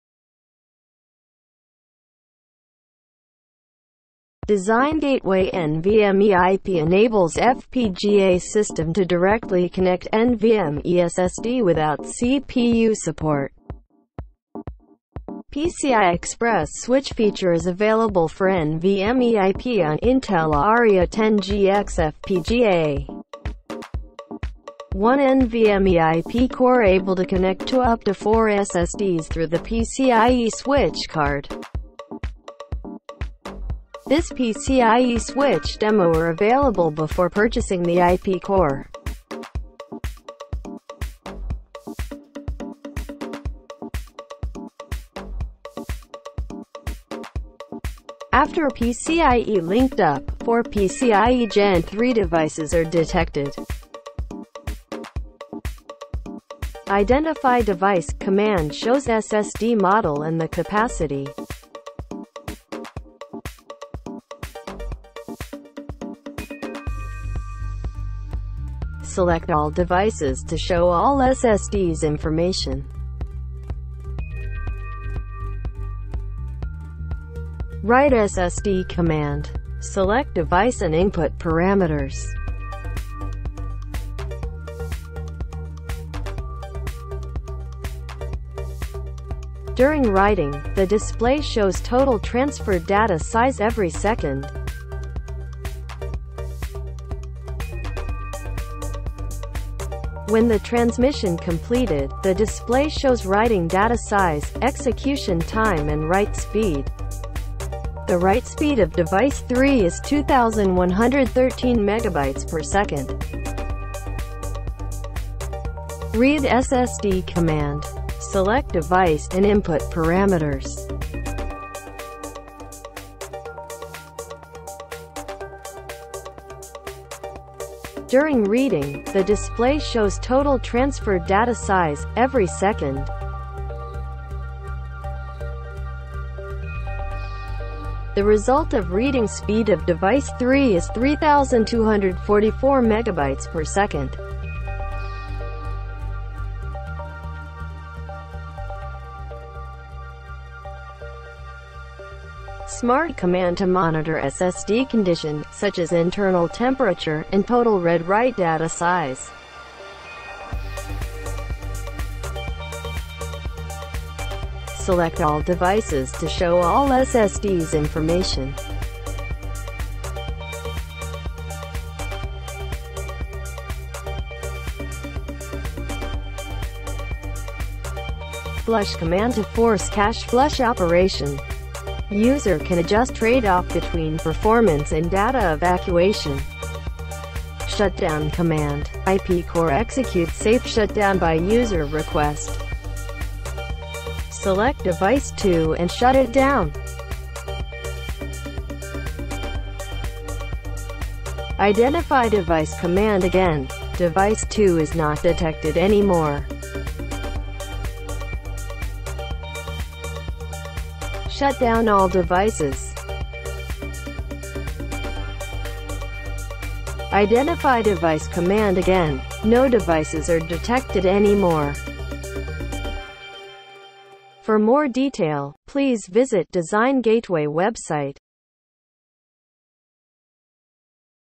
Design Gateway - NVMe IP core with PCIe Switch on Intel Arria 10 GX
PCI Express switch feature is available for NVMe IP core for Intel FPGA devices. One NVMe IP core able to connect to up to 4 SSDs through the PCIe switch card.